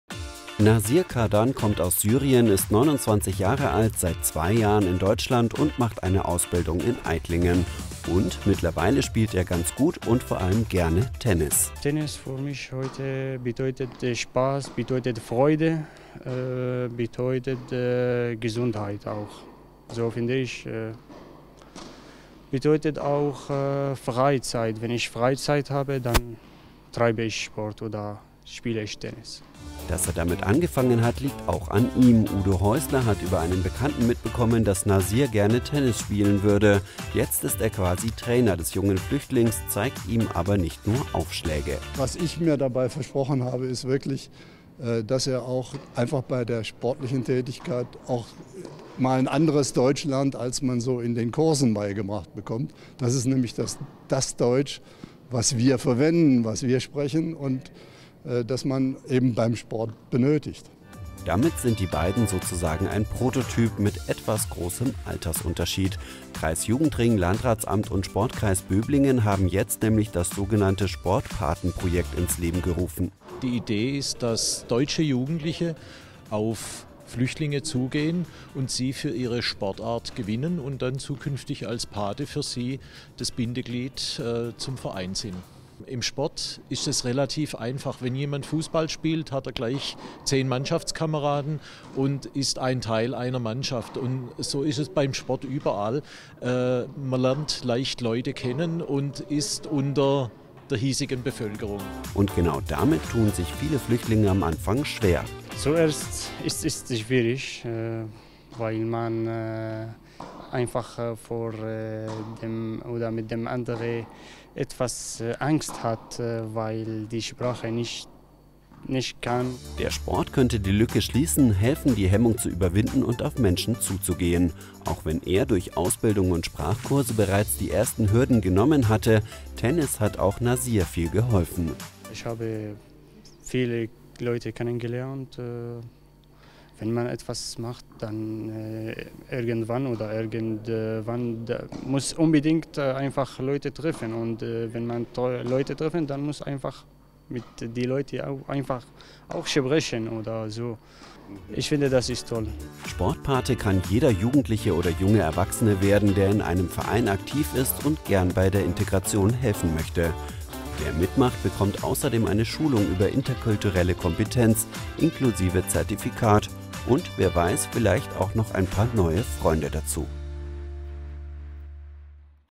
Integration spielerisch: Das Sportpaten-Projekt | Wir in BB
Sport hält fit, Sport ist gesund, und Sport hilf bei der Integration – oder soll es zumindest bald. Im Kreis Böblingen geht ein Sportpaten-Projekt an den Start, bei dem deutsche Jugendliche Flüchtlinge ansprechen und zum Vereinssport animieren sollen. Wie das funktionieren kann, kann man jetzt schon in Aidlingen sehen – wenn Nazir Kadan und Udo Häusler zusammen Tennis spielen.